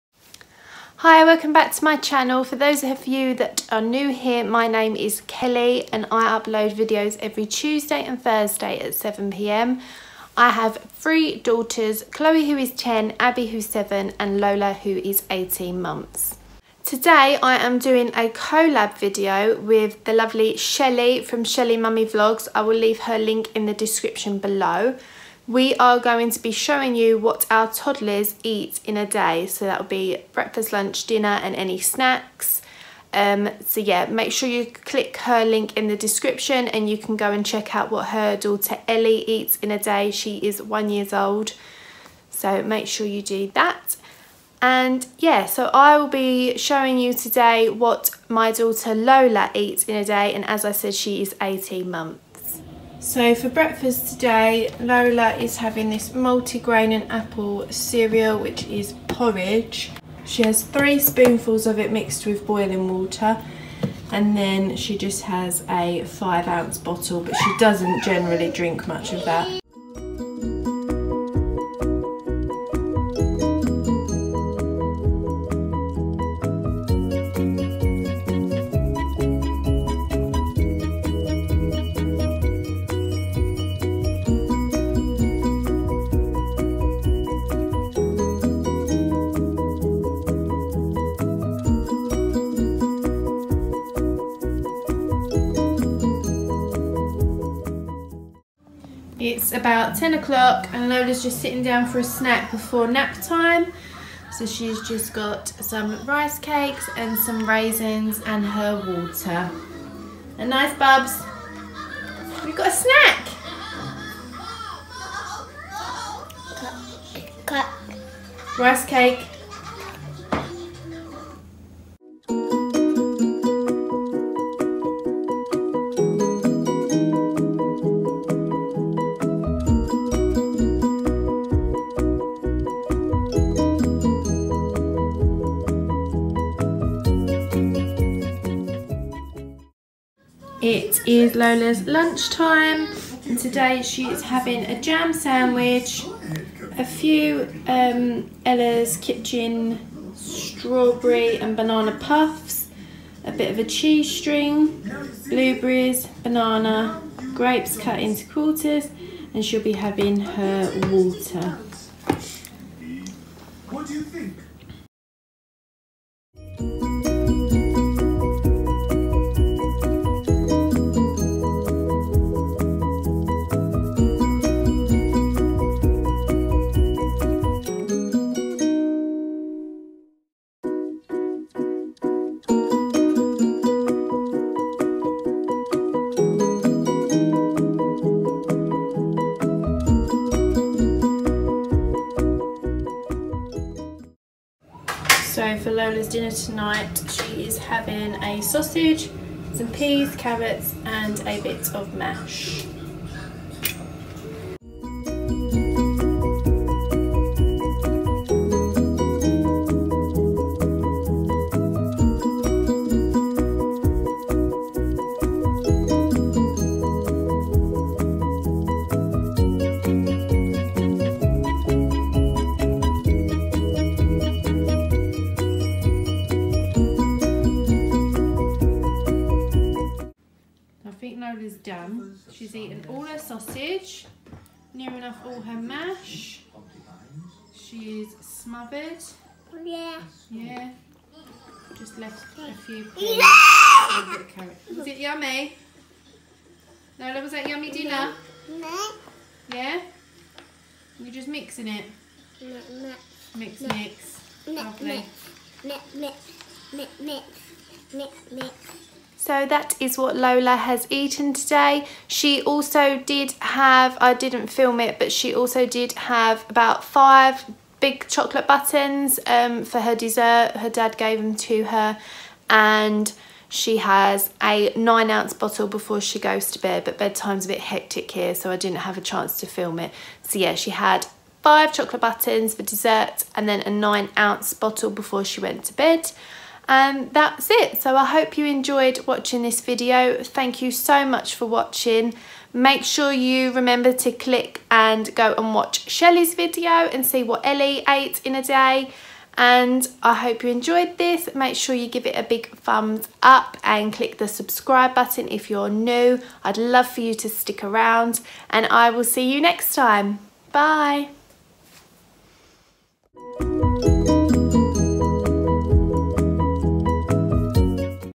What My Toddler Eats In A Day | Collab
What My Toddler Eats In A Day
Today I thought I’d share with you what my toddler eats in a day. This is a typical day with 3x meals, 2x bottles and a snack.
This is a collab video with Shelley and her daughter Ellie. Please make sure you click her link and watch her video of what Ellie eats in a day!